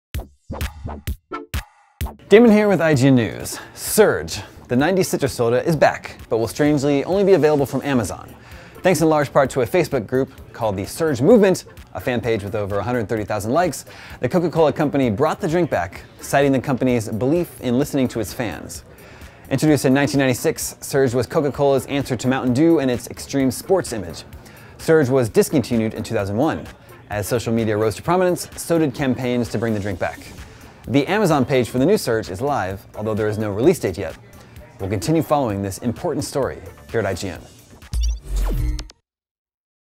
Back by Popular Demand, Surge Soda Returns - IGN News
Surge, the sweet citrus soda, is back, and is available exclusively through Amazon.